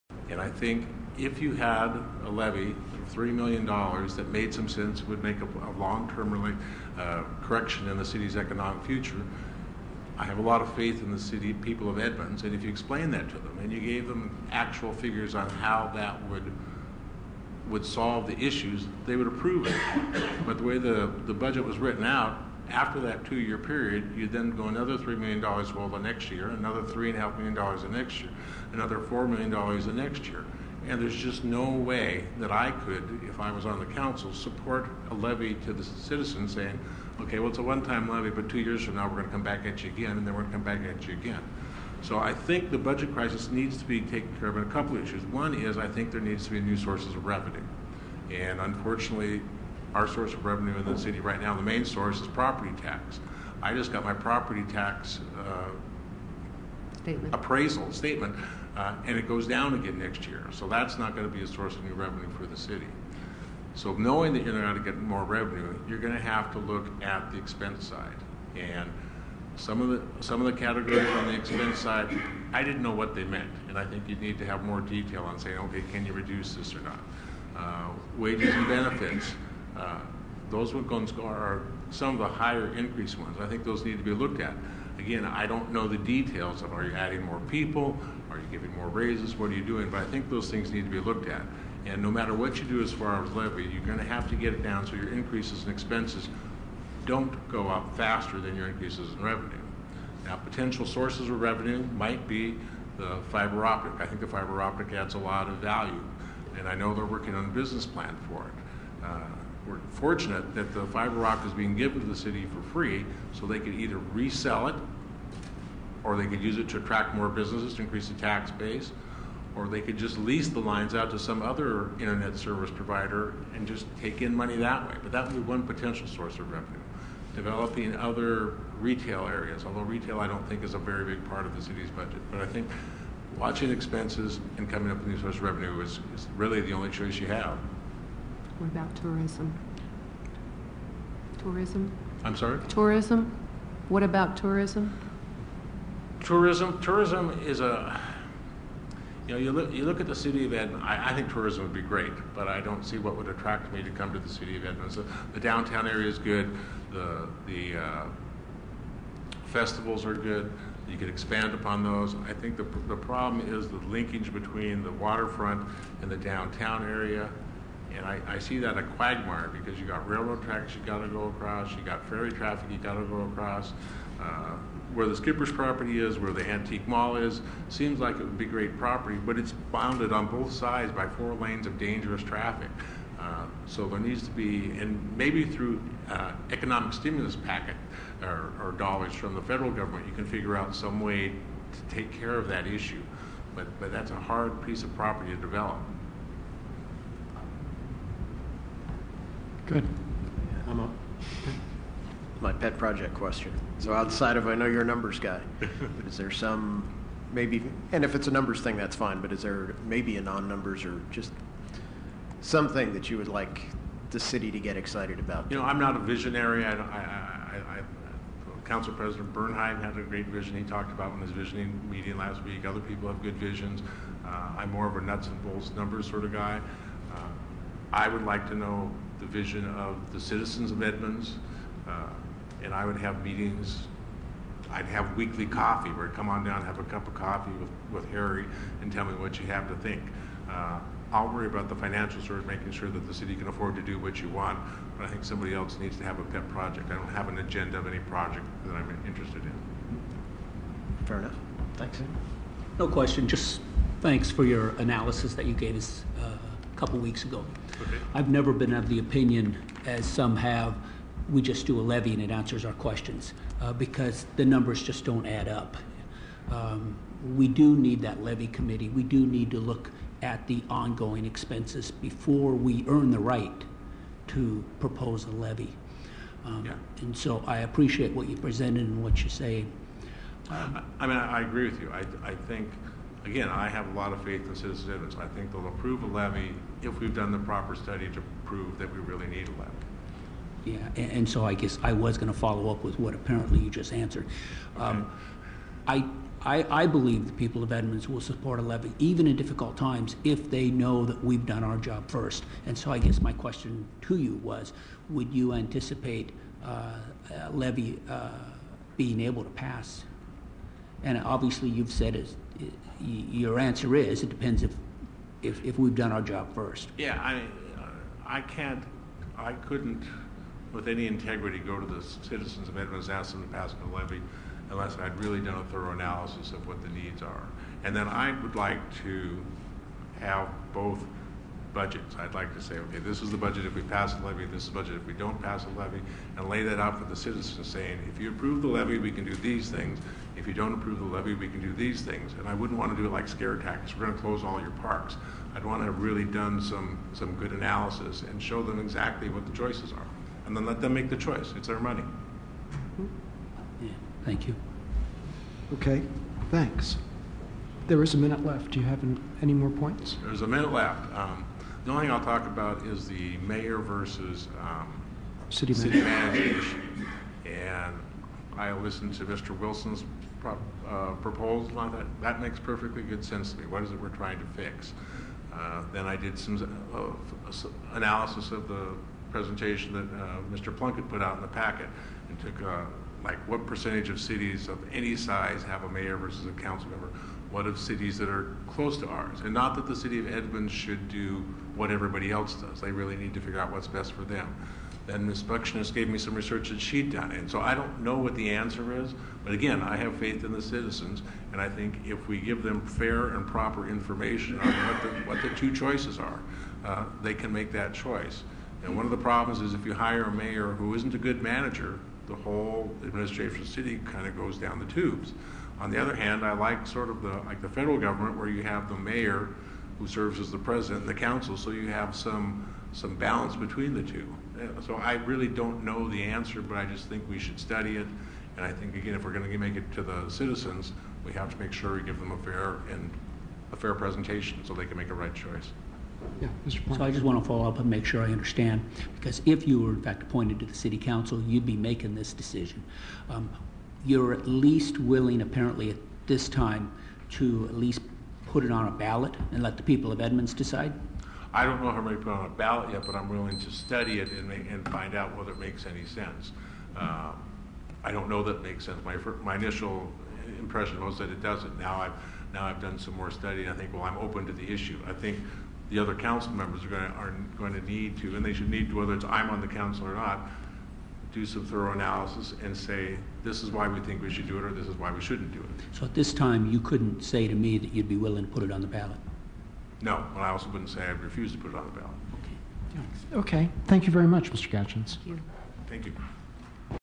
Harry Gatjens City Council Interview part 2
Harry Gatjens interviewed by Edmonds City Council regarding his application for appointment to Position #7 Part Two